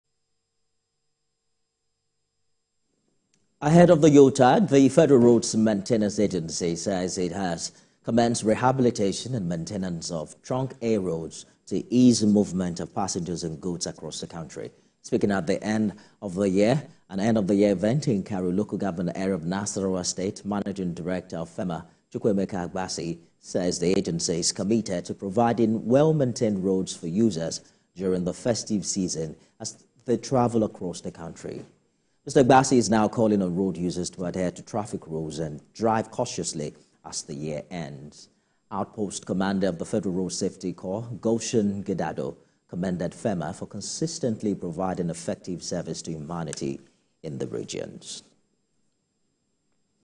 FERMA Commences Road Rehabilitation Ahead Of Yuletide
Ahead of the yuletide, the Federal Road Maintenance Agency says it has commenced rehabilitation and maintenance of Trunk A roads to ease movement of passengers and goods across the country.
Speaking at an end of the year in Karu Local Government Area of Nasarawa state, managing director of FERMA, Chukwuemeka Agbasi, says the agency is committed to providing well-maintained roads for users during the festive season as they travel across the country.